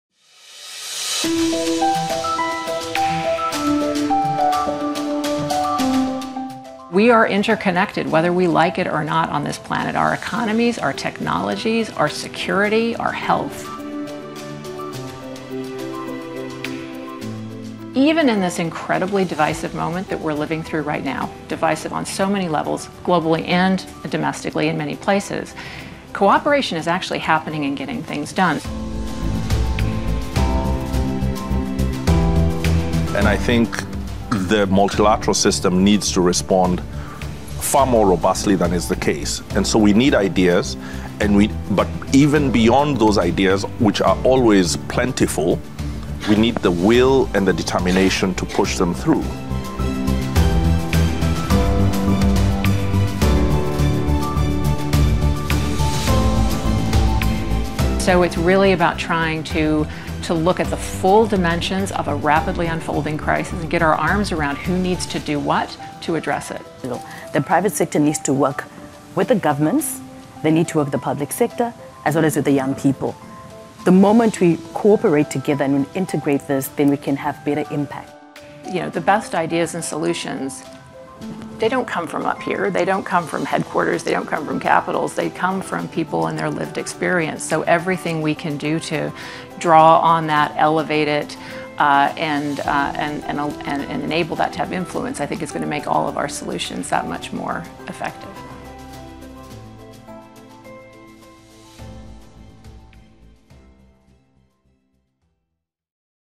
FT Live: Cooperating to Solve Shared Challenges
The world needs a new level of cooperation. From public-private partnerships to governments engaging with young people, we must develop solutions based on lived experiences. The answer to the challenges of the moment won’t come from just one place. It's going to take all of us.

Watch more in this video featuring Elizabeth Cousens, UN Foundation President and CEO, Ambassador Martin Kimani, Permanent Mission of the Republic of Kenya to the UN, and Chido Mpemba, the African Union Commission Special Envoy on Youth.

For more info on the strategic preview of UN General Assembly 2022, co-hosted by UN Foundation and the Financial Times,